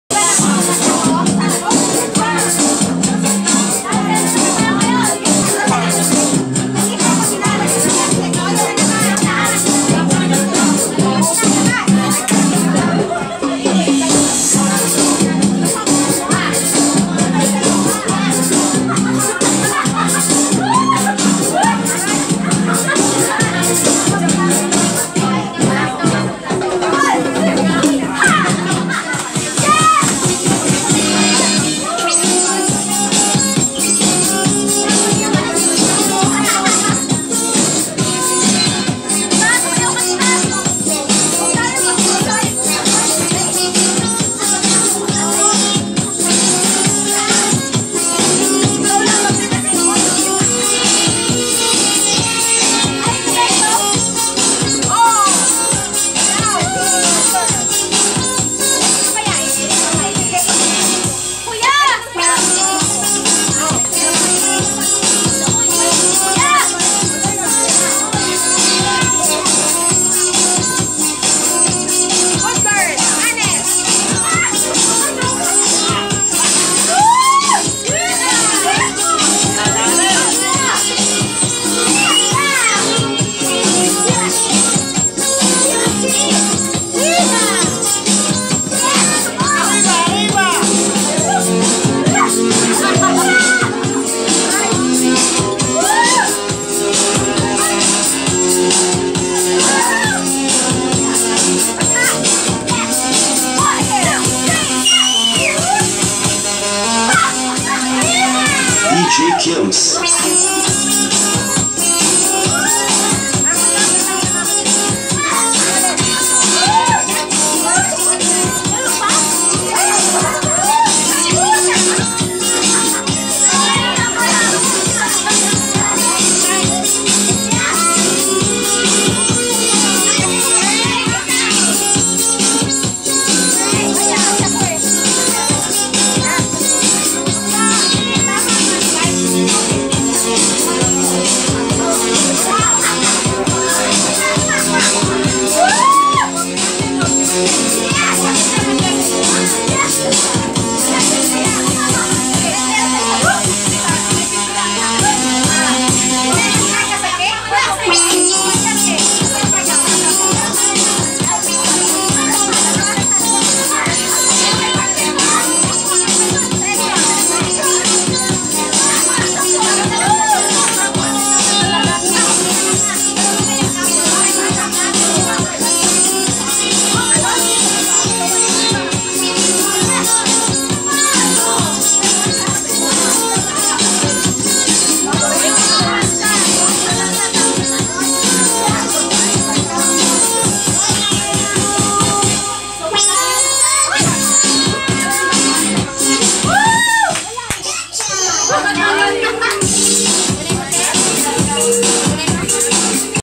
LUZVIMIN BAND PARTY
Members enjoy dancing on the yacht floor